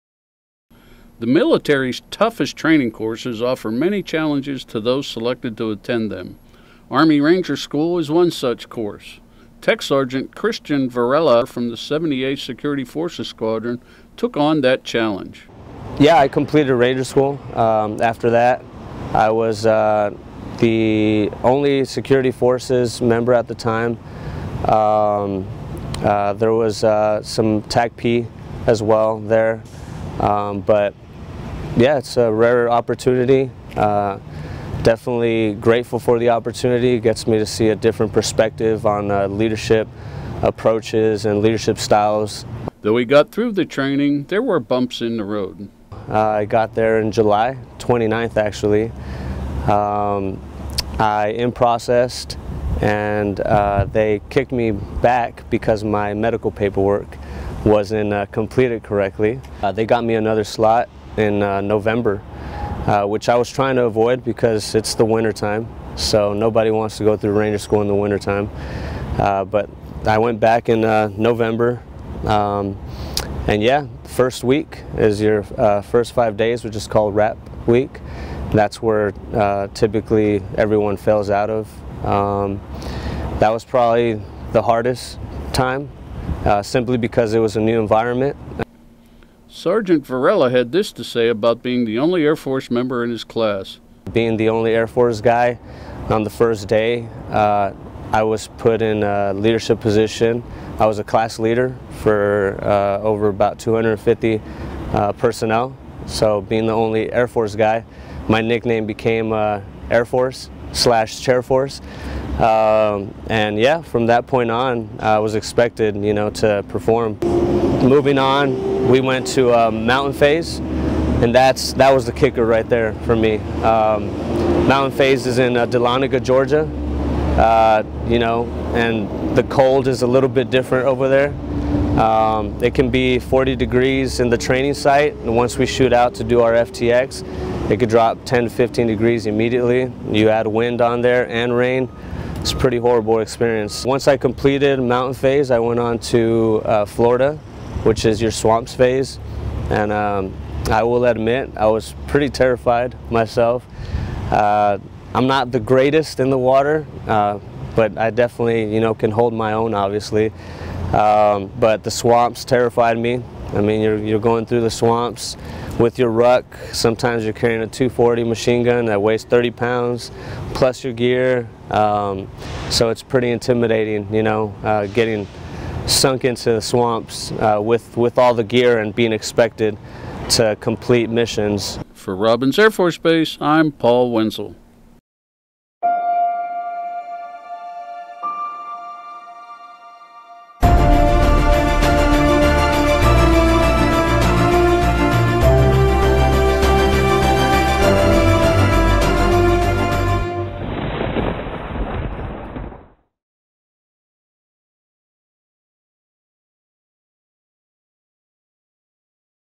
Air Force Ranger
The military’s toughest training courses offer many challenges to those selected to attend them. Army ranger school is one such course. TSgt. Christian Varela, a Security Forces member from the 78th Security Forces Squadron took on the challenge.  Here is a little insight into what he went through.  (U. S. Air Force video by Paul Wenzel)